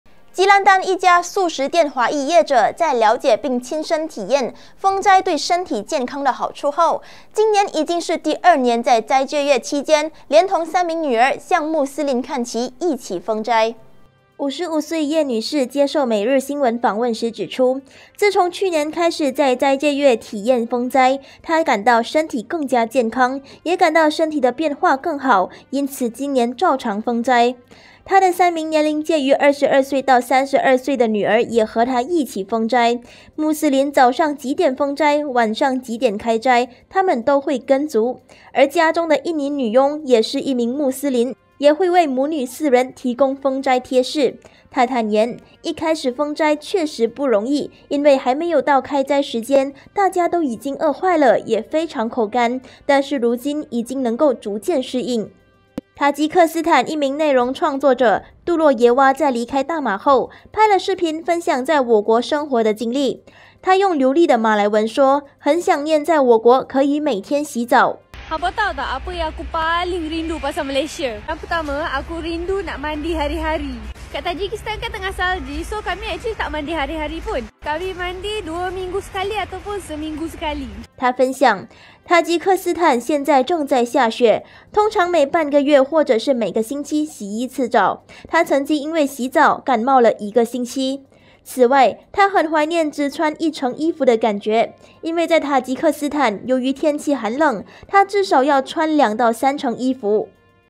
【感到身体更健康 华妇与3女儿体验斋戒】
吉兰丹一家素食店华裔业者，在了解并亲身体验封斋对身体健康的好处后，今年已是第二年在斋戒月期间，连同3名女儿向穆斯林看齐，一起封斋。